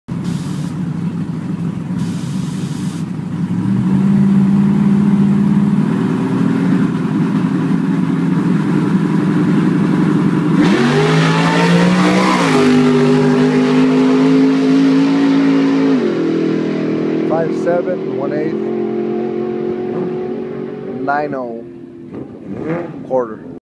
Spike Performance testing spike @ ERT 07/24/13 9.00 @ 149
Went out for some test hits in spike to get her ready for some racing.  Ran 10.2 on motor in hot weather then sprayed and went 9.00 @ 149